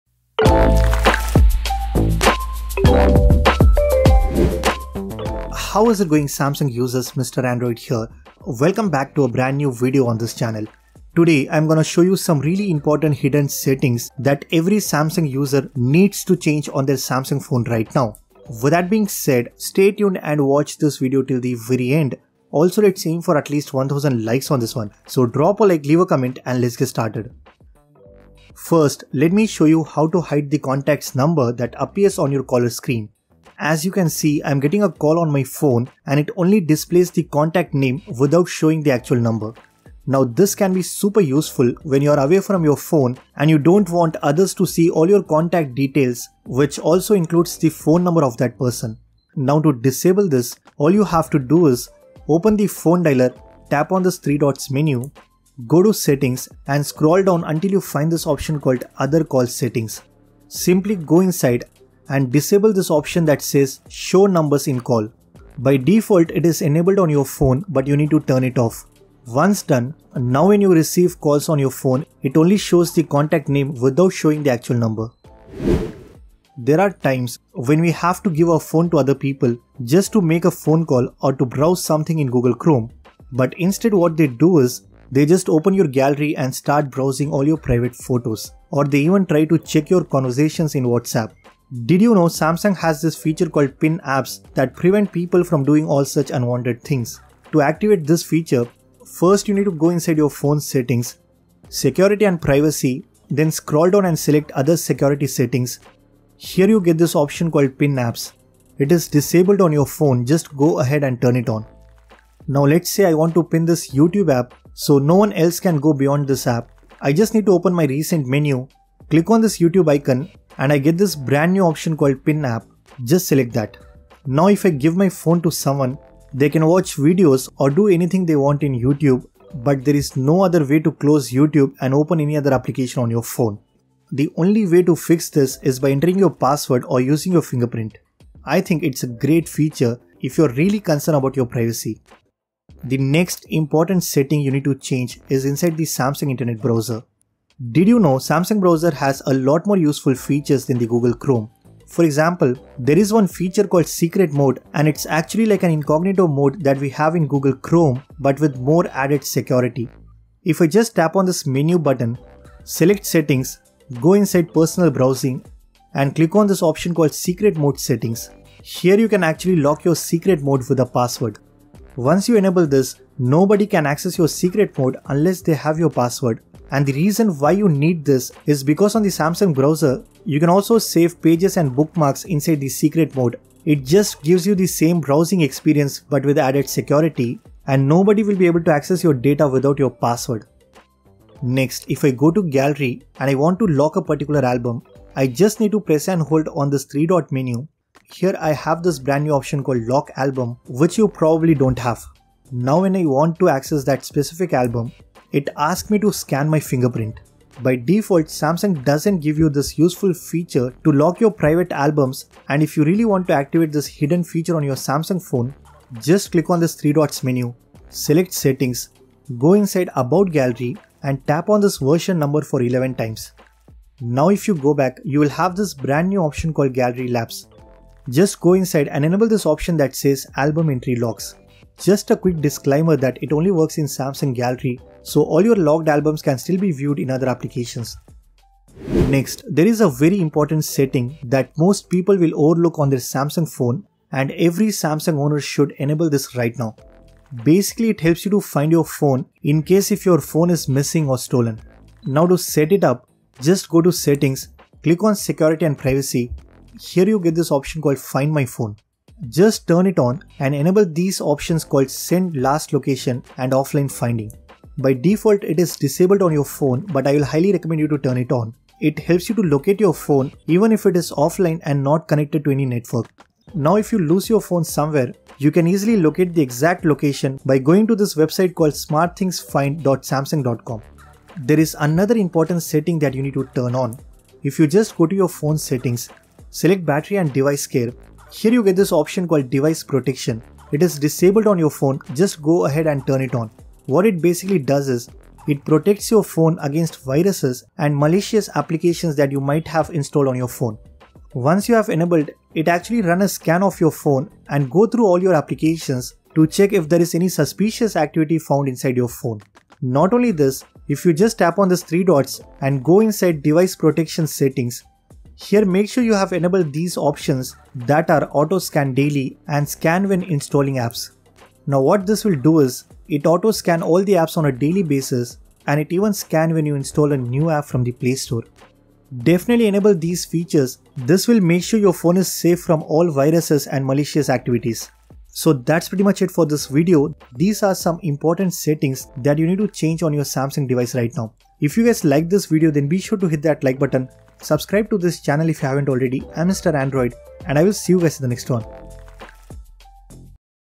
CHANGE These IMPORTANT HIDDEN Samsung Settings BEFORE IT'S TOO LATE - You Should KNOW!
In this video, I will show you some really IMPORTANT HIDDEN Samsung settings you need to change right now on your Samsung galaxy device.

Timestamps :
00:00 intro
00:28 1.
01:16 2.
02:23 3.
03:11 4.
03:59 5.
04:41 6.
05:37 outro